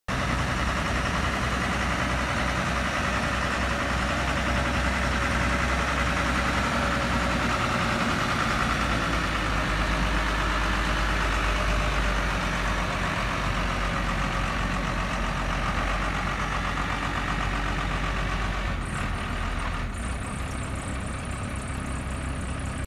1995 MOBIL SWEEPER M9 For Sale
1995 Mobil M9D street sweeper
In good working condition
Located and can be seen at Leavinworth MN